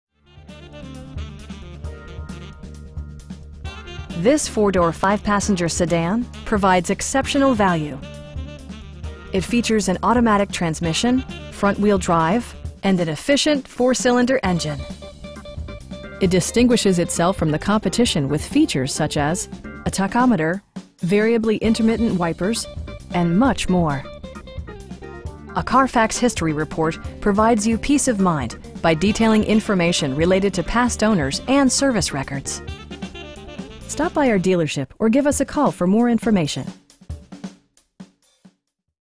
1998 Saturn SL1 Base in Orlando, FL 32837
11051 S Orange Blossom Trail

This 4 door, 5 passenger sedan provides exceptional value! It features an automatic transmission, front-wheel drive, and an efficient 4 cylinder engine. It distinguishes itself from the competition with features such as: a tachometer, variably intermittent wipers, front bucket seats, tilt steering wheel, and much more. A Carfax history report provides you peace of mind by detailing information related to past owners and service records. We pride ourselves in consistently exceeding our customer's expectations. Stop by our dealership or give us a call for more information.